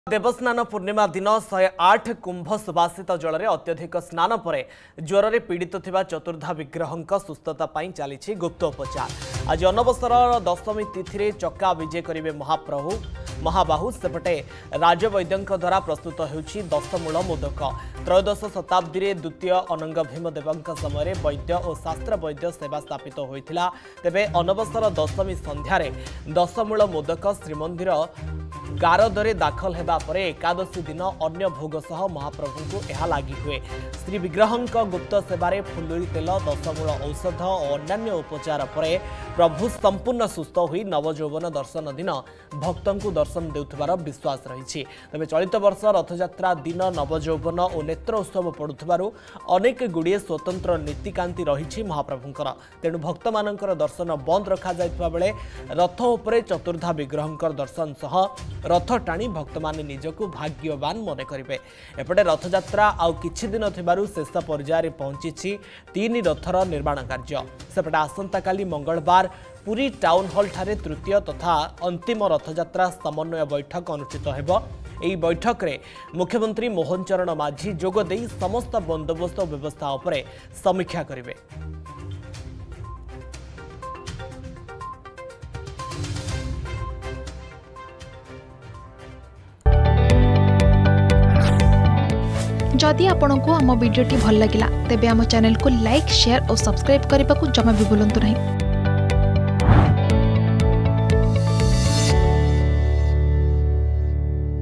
ଚତୁର୍ଦ୍ଦାବିଗ୍ରହଙ୍କର ସୁସ୍ଥତା ପାଇଁ ଚାଲିଛି ଗୁପ୍ତ ଉପଚାର || AnaSara || Puri
ଦେବସ୍ନାନପୂର୍ଣ୍ଣିମା ଦିନ ୧୦୮ କୁମ୍ଭ ସୁବାସିତ ଜଳରେ ଅତ୍ୟଧିକ ସ୍ନାନ ପରେ ଜ୍ଜରରେ ପୀଡ଼ିତ ଥିବା ଚତୁର୍ଦ୍ଦାବିଗ୍ରହଙ୍କର ସୁସ୍ଥତା ପାଇଁ ଚାଲିଛି ଗୁପ୍ତ ଉପଚାର। ଆଜି ଅନବସରର ଦଶମୀ ତିଥିରେ ଚକାବିଜେ କରିବେ ମହାବାହୁ।